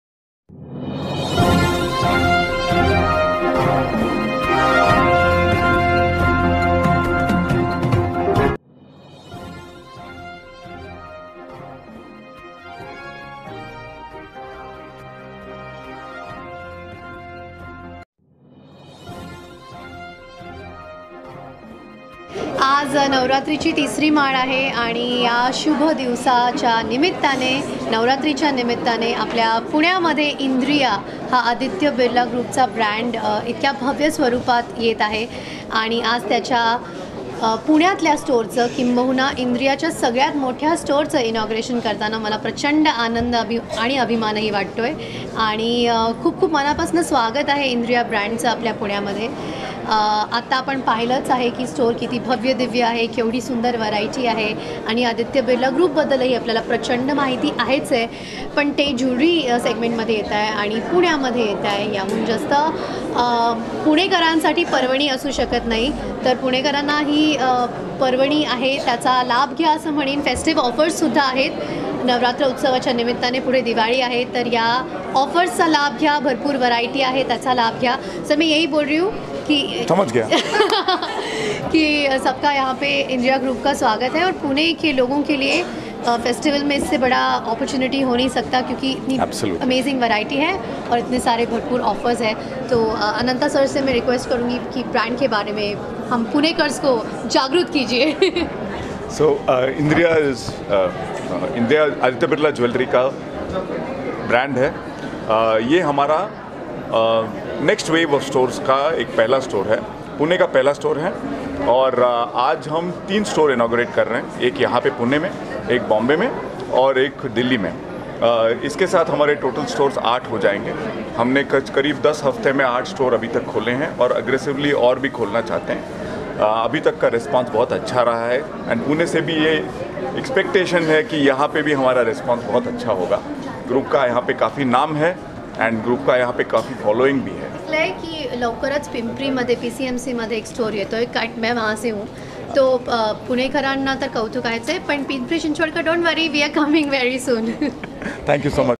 आदित्य बिरला ग्रुपच्या इंद्रिया’चे पुण्यातील पहिले दालन सुरू ..
पुण्यातील पहिल्या इंद्रिया दालनाच्या साथीने उत्कृष्ट दागिन्यांचे विश्व अनुभवा - अभिनेत्री सोनाली कुलकर्णी